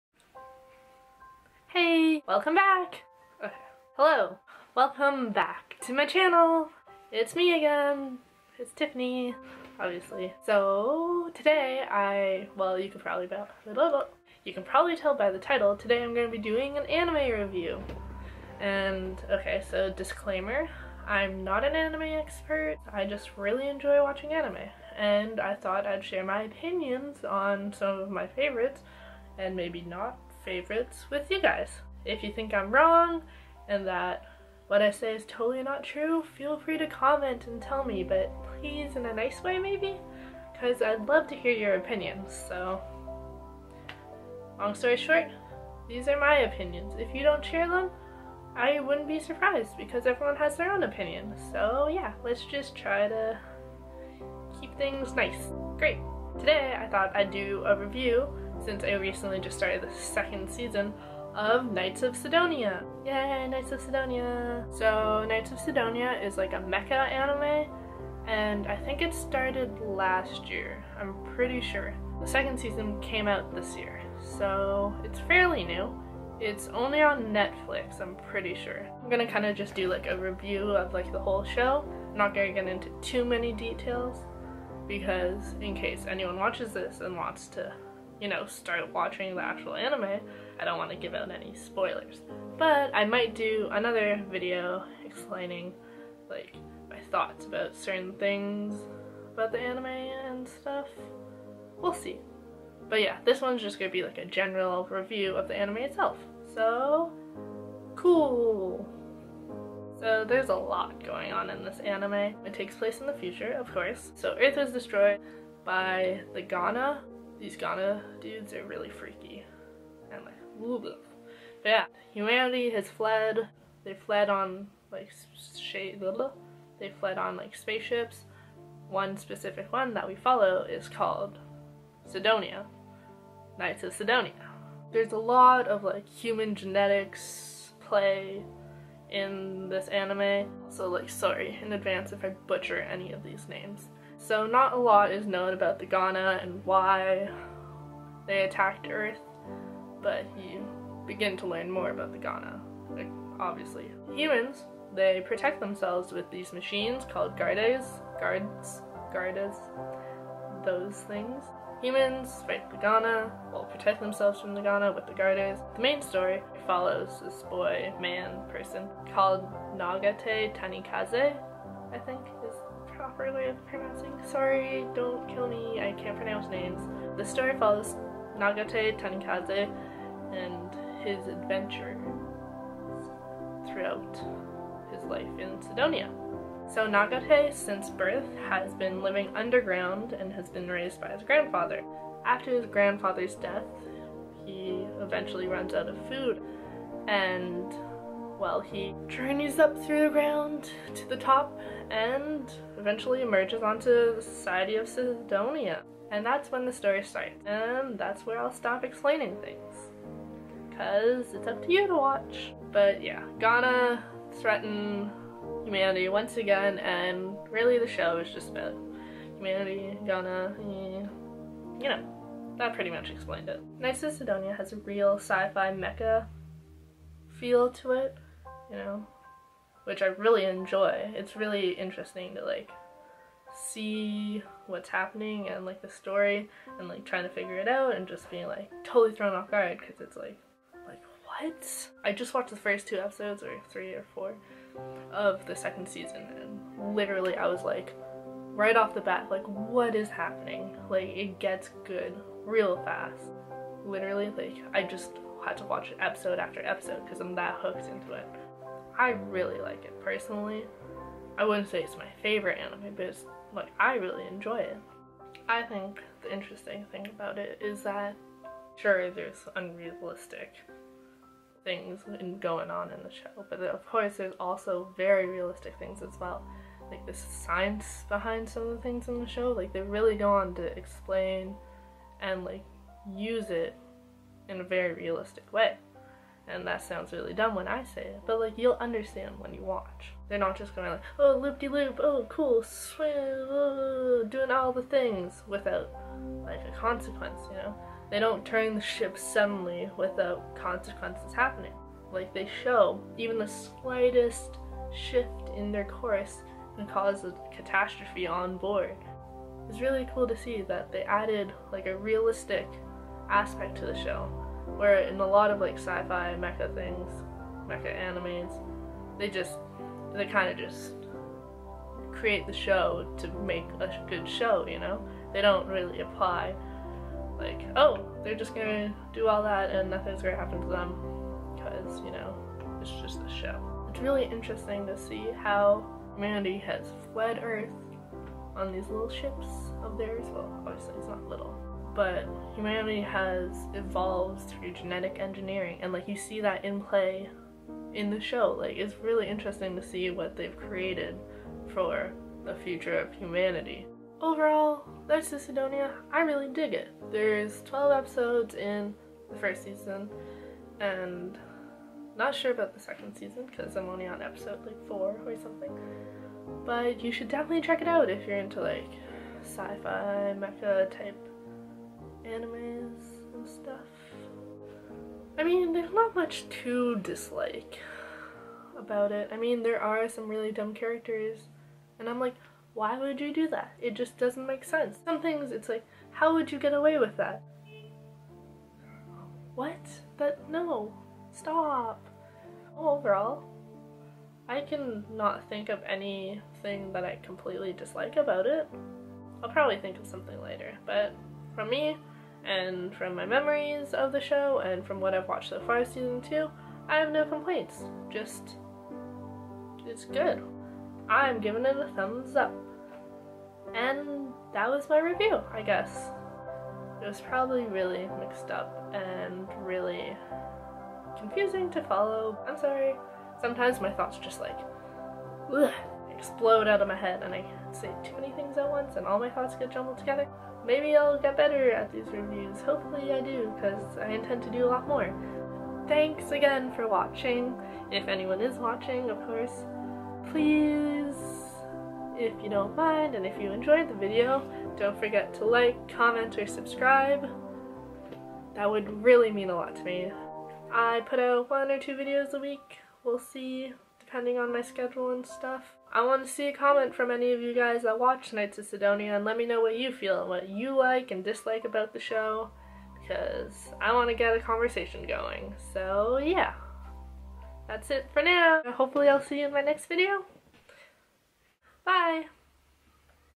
ANIME REVIEW: Knights of Sidonia!
This is my first anime review i will be doing on my channel! Since i started the second season recently i decided to do one on Knights of Sidonia! Just a reminder this is a general review, not getting into too much details :) Don't want to spoil anything! I just love anime and thought i would share my opinions :)